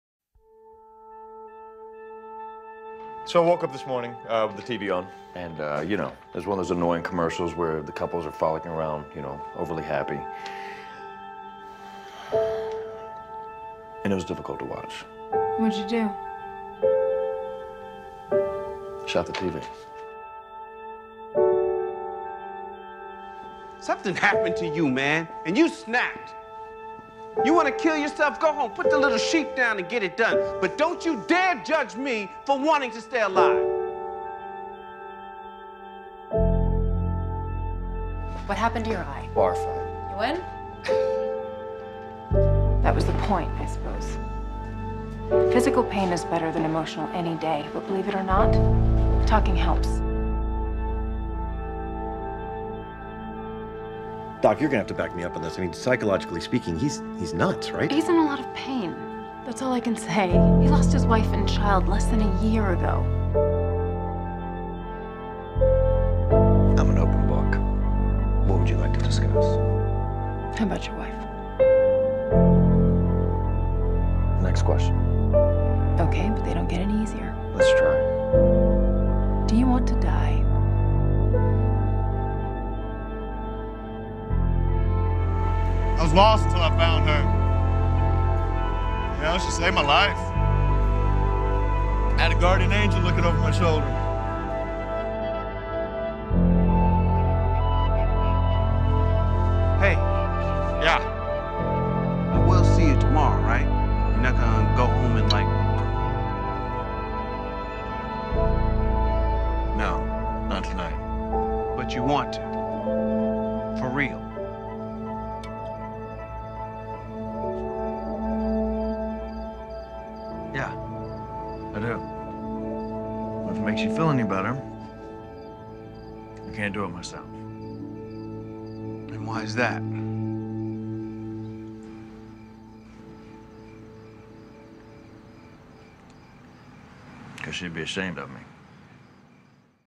Martin Riggs - 'Cause she'd be ashamed of me
Just a little video about Martin R.
Sorry for the glitch at 1:28
Coloring by : xKittyDream / xKittyTutorials
Xoxo.